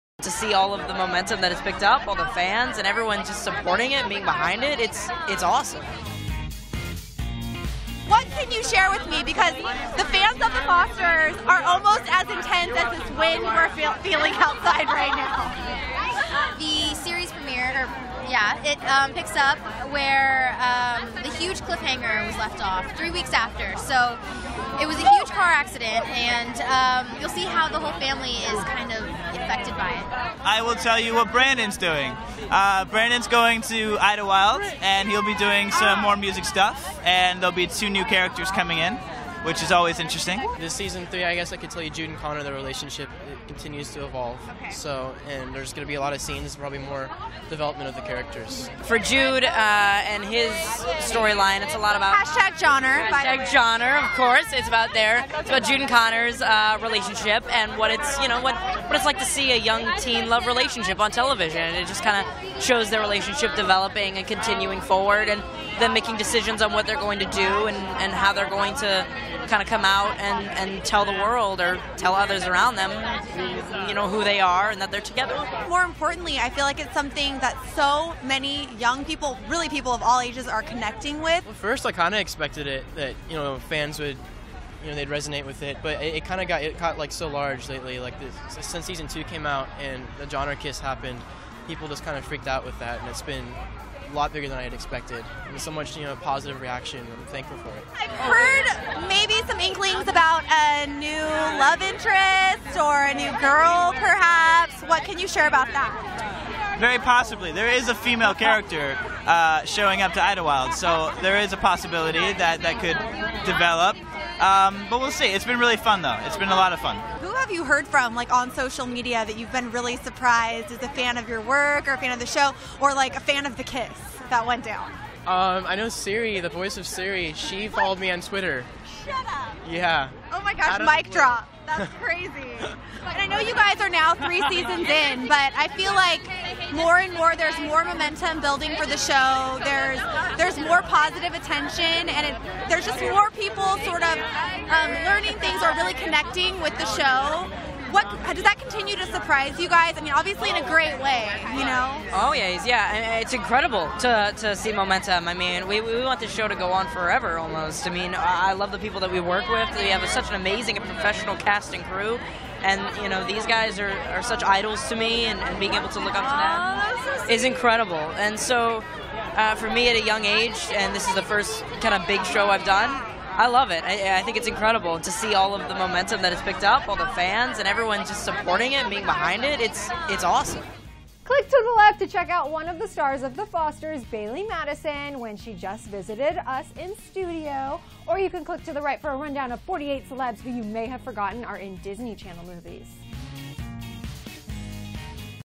[LEGENDADO] David Lambert, Hayden Byerly, Cierra Ramirez e Gavin MacIntosh no
Durante o Radio Disney Music Awards, premiação americana que aconteceu dia 26 de abril, em Los Angeles, David Lambert, Hayden Byerly, Cierra Ramirez e Gavin MacIntosh falaram sobre a terceira temporada de "The Fosters" em entrevista ao ClevverTV.

A terceira temporada de "The Fosters" estreia dia 8 de junho!